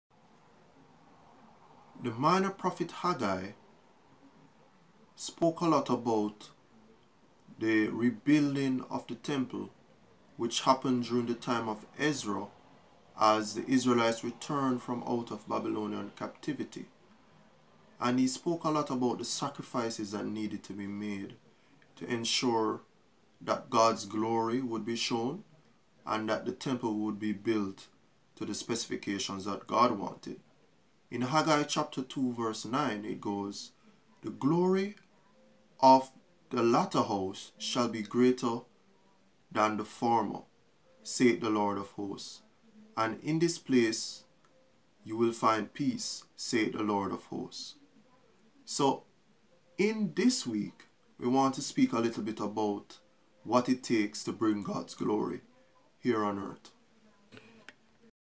THE GLORY IS COMING: PART 1 - Introduction
THE GLORY IS COMING: PART 1 - INTRODUCTION // "The glory of this latter house shall be greater than of the former, saith the Lord of hosts: and in this place will I give peace, saith the Lord of hosts." Haggai 2:9

✅ Follow 👉 Joneil Alcock 👈 for more.